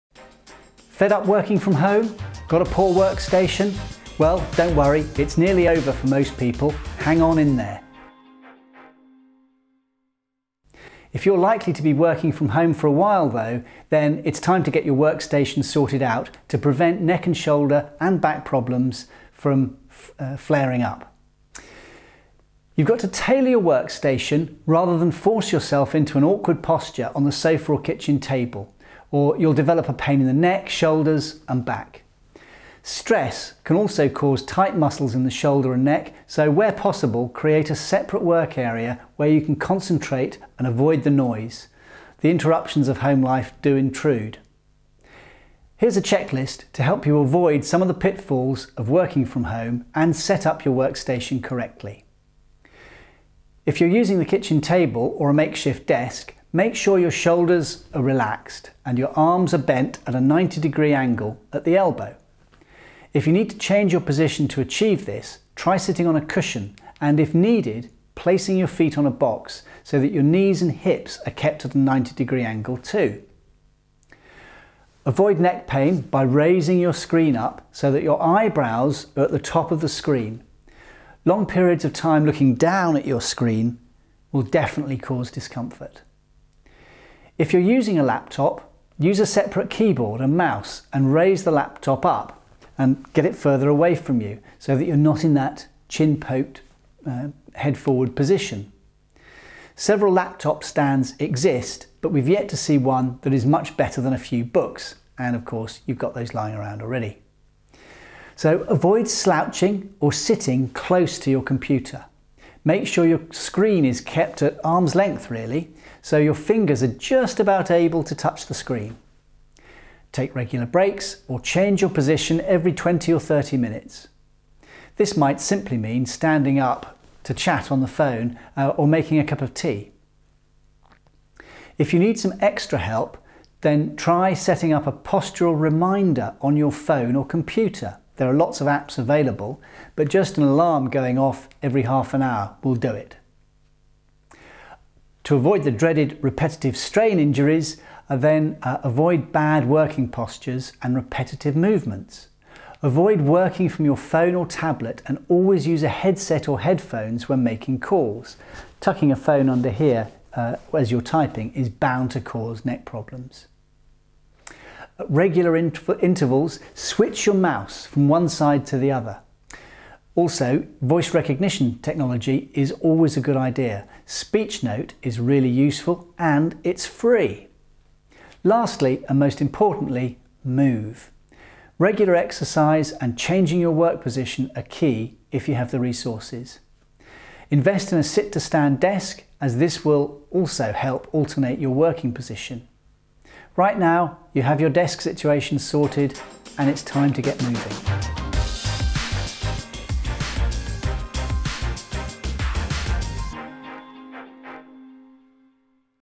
How to set up workstation at home
You must tailor your workstation to you rather than force yourself into awkward postures on the sofa or kitchen table or you will develop pain in the neck, shoulders and back. Stress can also cause tight muscles in the shoulder and neck so, where possible, create a separate work area where you can concentrate and avoid the noise and interruptions of home life.

Here’s a checklist to help you avoid some of the pitfalls of working from home and set up your workstation correctly.